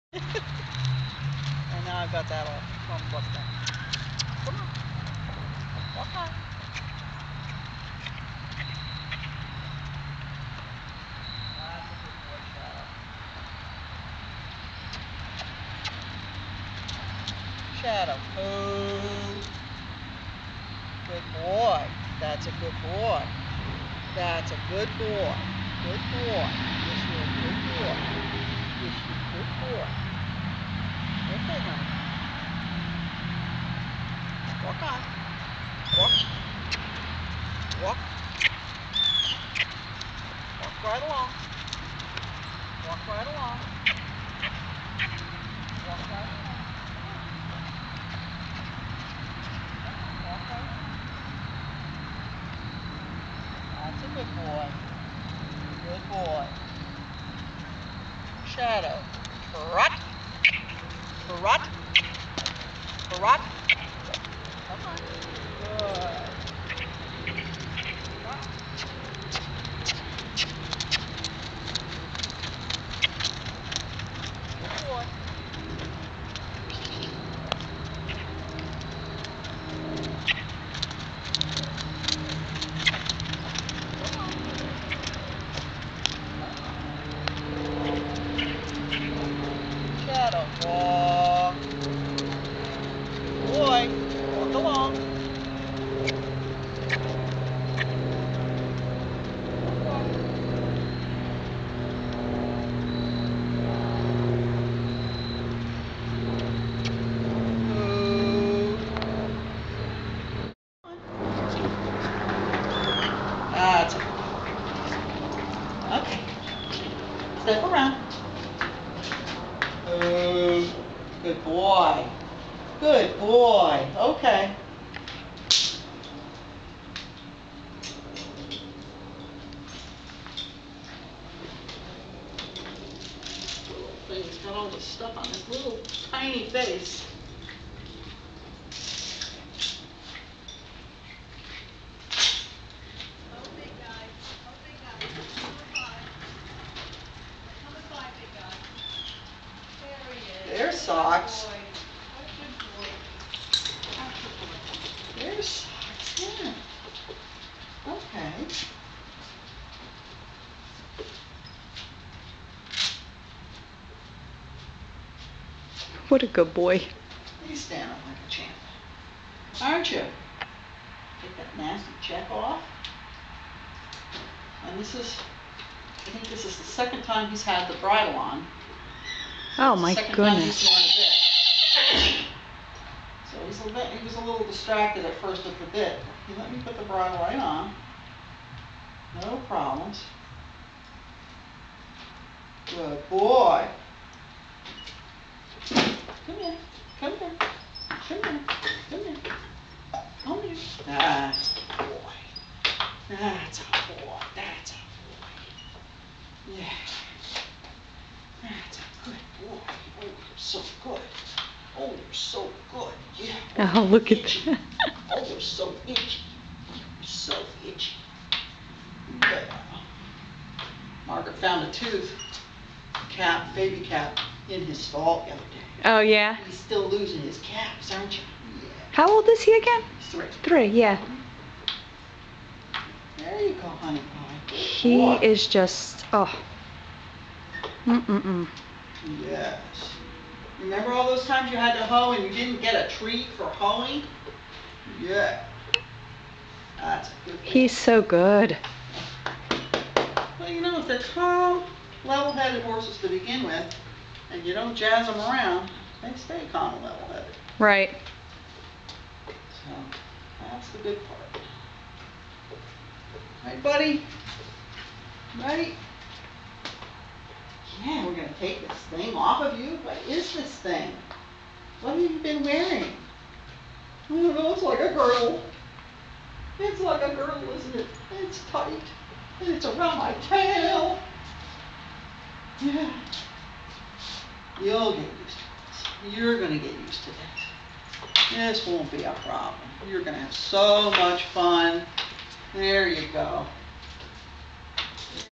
Sunswepts Five O'Clock Shadow
Training clips of Sunswepts Five O'Clock Shadow, a 3year old miniature horse owned by Pam Corsa.